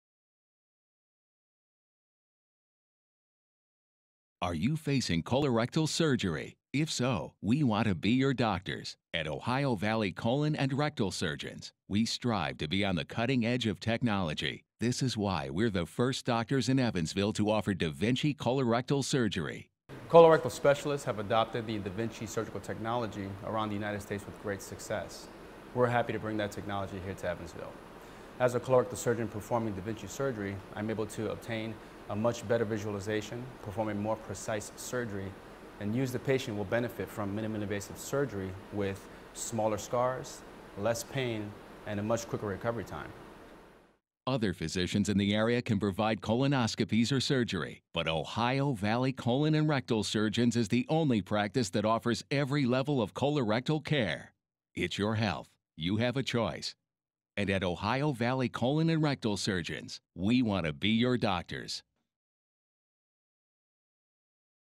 da Vinci Colorectal Surgery at Ohio Valley Colon & Rectal Surgeons in Evansville IN
Ohio Valley Colon & Rectal Surgeons are the first in Evansville to offer da Vinci Surgery for Colon and Rectal Conditions, including colon and rectal cancers, diverticulitus and Chron's disease.  Other doctors can offer colonoscopies or surgery, but only Ohio Valley Colon & Rectal Surgeons offers you all of these services.  At Ohio Valley Colon & Rectal Surgeons, we want to be your doctors.

For more information about da Vinci Colorectal Surgery, please

da Vinci Colorectal Surgery

If your doctor recommends surgery to treat a colorectal condition, you may be a candidate for minimally invasive da Vinci Surgery. Using state-of-the-art technology, the da Vinci System enables doctors to perform delicate and complex operations through a few tiny incisions with breakthrough vision, precision, dexterity and control.

da Vinci Surgery is used to treat such colorectal conditions as: colon cancer, rectal cancer, diverticulitis, and inflammatory bowel disease (ulcerative colitis and Crohn's disease). Surgery to remove all or part of the colon is known as a colectomy. Rectal cancer surgery is known as a low anterior resection.

da Vinci offers patients facing colon surgery (right, left or sigmoid colectomy) such potential benefits as:

Low blood loss1
Quicker return to bowel function1
Quicker return to diet1
Short hospital stay1
da Vinci offers patients facing rectal cancer surgery (low anterior resection and APR) such potential benefits as:

Excellent clinical outcomes for cancer control3
Quick return of bowel function3
Fast return to diet3
Less blood loss4
Shorter hospital stay4
Fast recovery time3
The da Vinci System is a state-of-the-art surgical platform with 3D, high-definition vision and patented surgical instruments that takes surgery beyond the limits of the human hand. By overcoming the challenges of traditional open and laparoscopic surgery, da Vinci is changing the experience of surgery for people around the world.